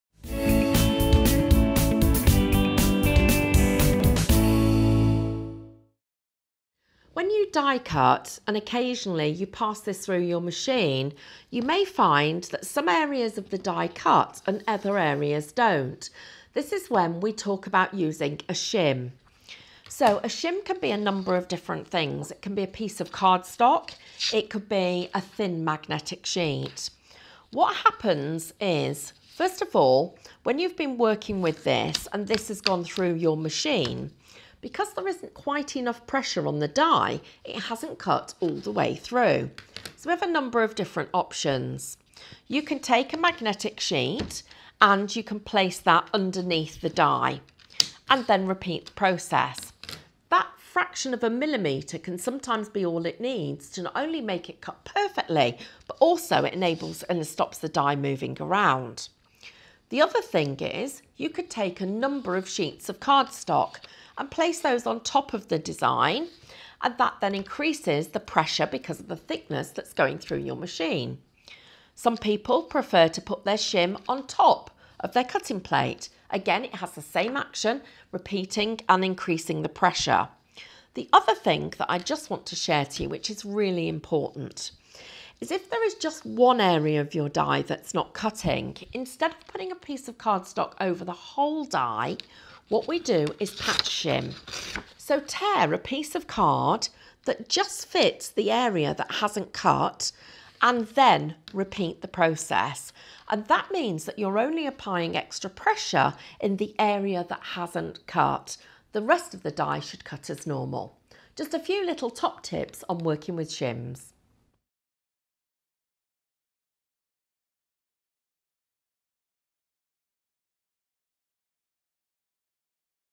Working with Shims | Hobbycraft
If you find your dies aren't quite cutting all the way through your cardstock, shims are a quick and easy way to eliminate this problem. Learn how to work with shims with this how to.